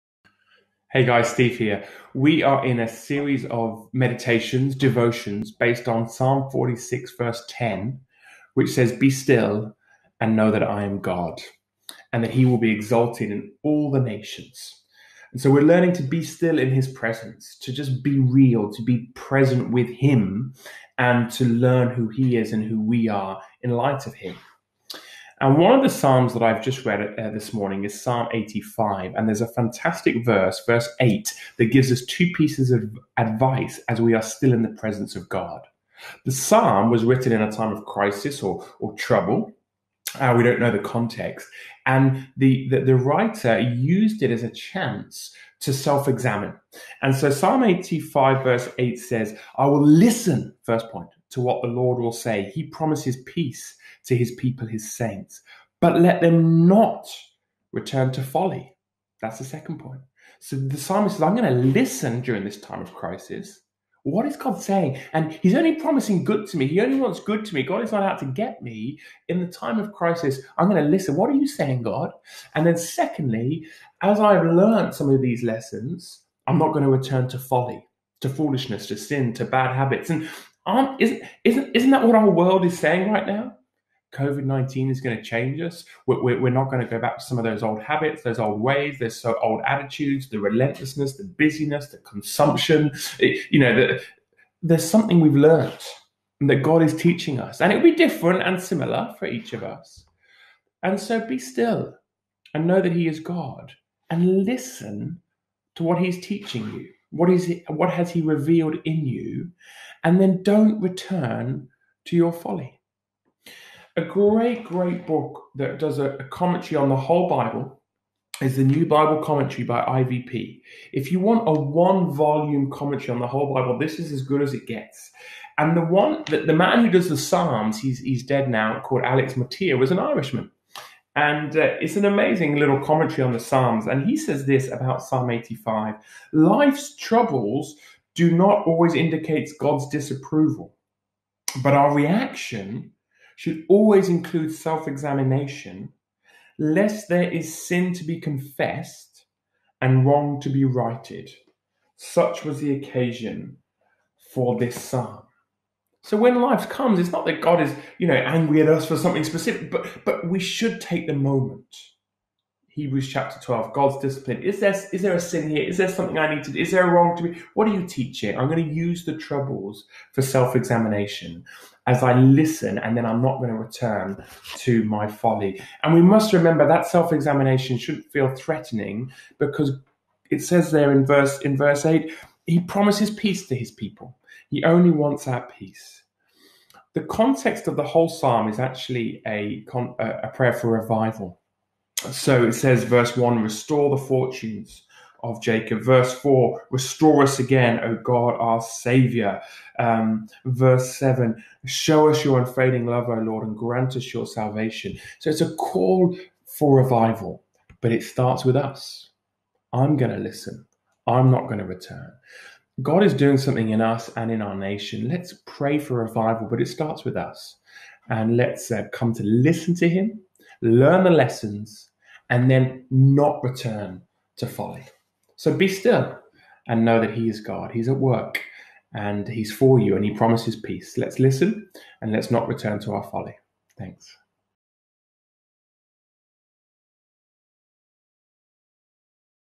Psalm 85 - Listening and not returning to folly #10
The psalmist is praying for revival, but knows it starts with him, and as verse 8 says, that he must (a) first listen to what God is saying and then (b) ensure that once the disaster has passed that he doesn't return to his folly. How apt is this for us during our challenging season.